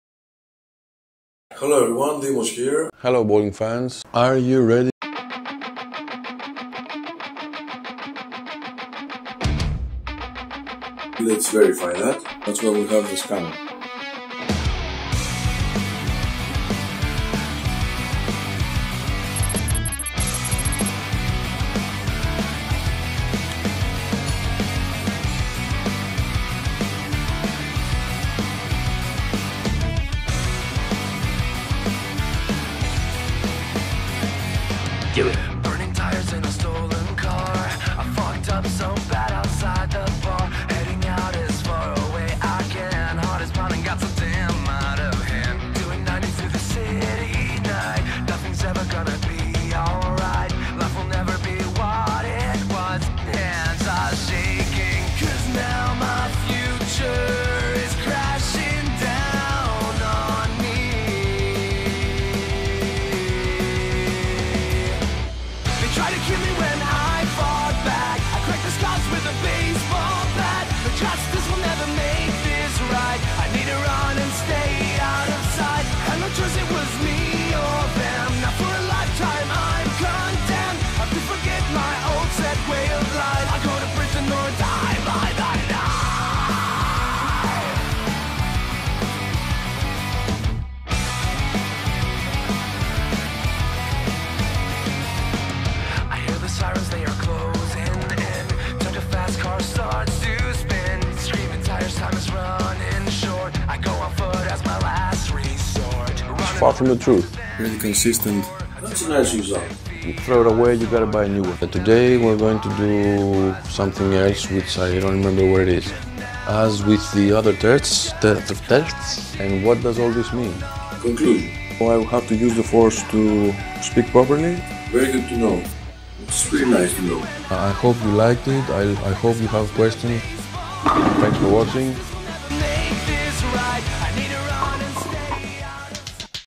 What the channel is about ! (Short Trailer ! )
Independent bowling science tests, reviews and experiments :)
For a better experience, watch with English subs ON (created by me 💪 )